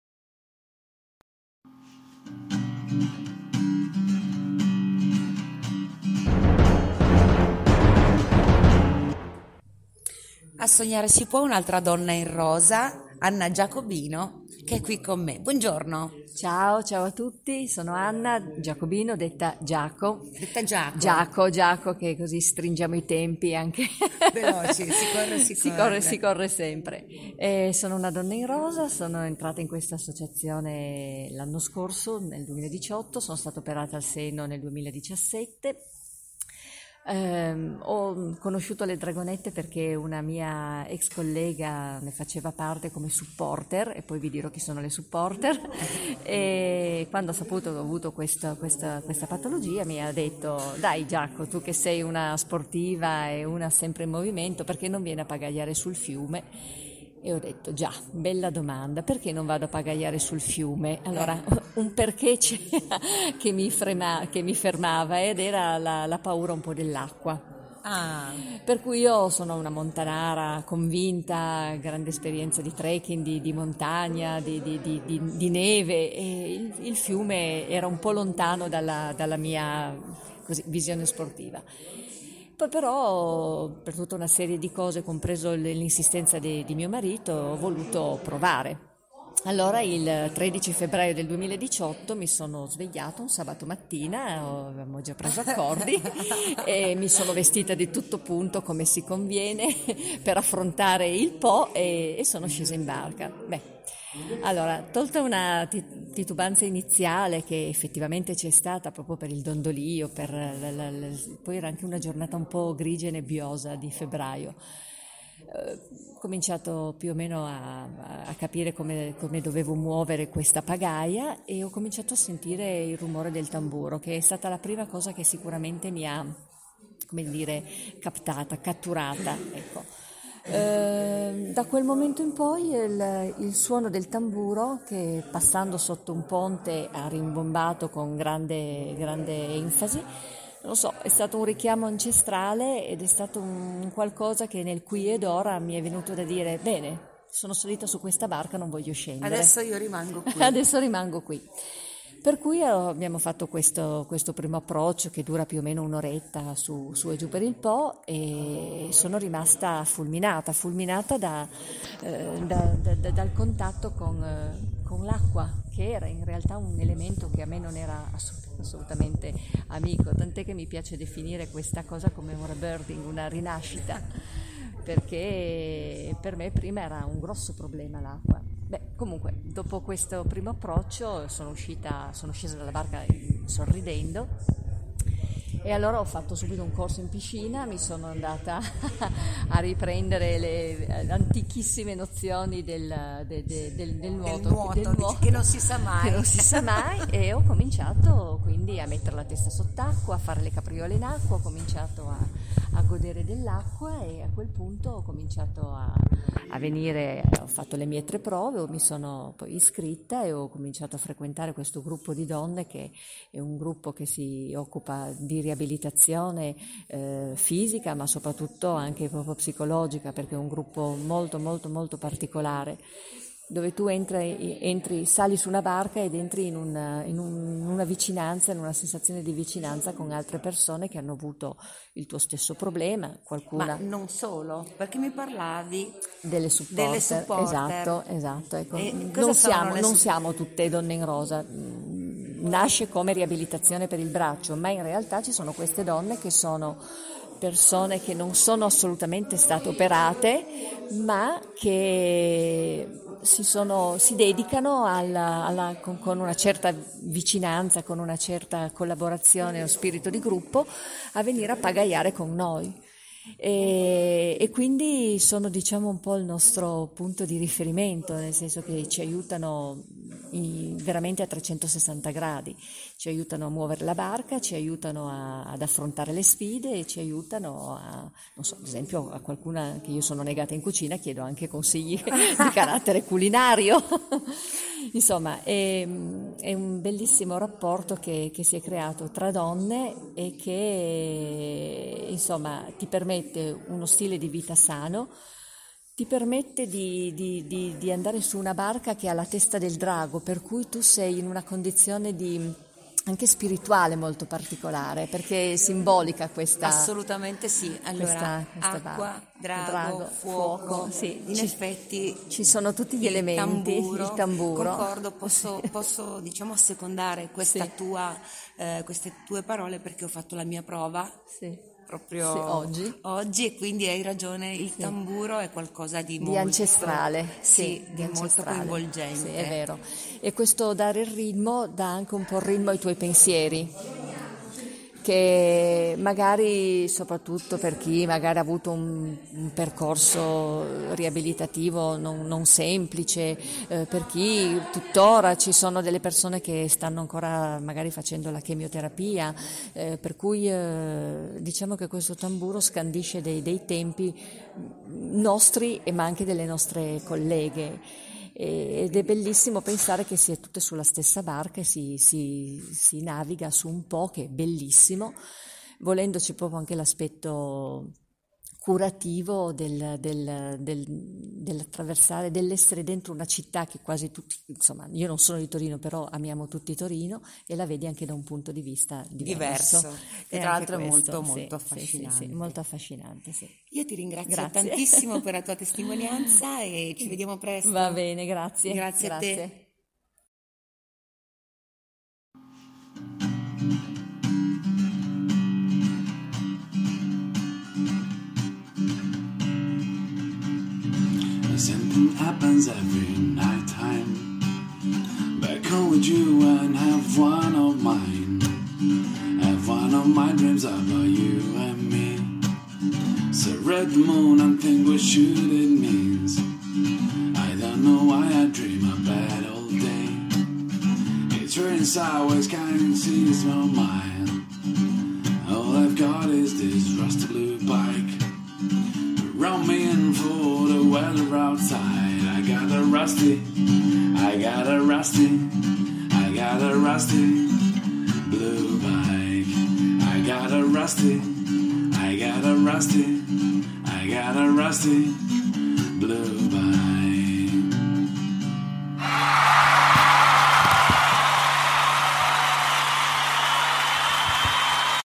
Sognare si può - Anna Giacobino, Giaco Per Gli Amici Giaco Open Day - Dragonette Torino Onlus
Una donna in Rosa, Anna Giacobino, Giaco per gli amici, mi racconta una storia. La sua.
Quella di una donna forte, colta che ama la 'montagna' e che temeva l'acqua ma si avvicina al fiume al tamburo alla pagaia e ... una splendida storia, raccolta durante l'Open Day - Dragonette Torino Onlus
Buon Ascolto!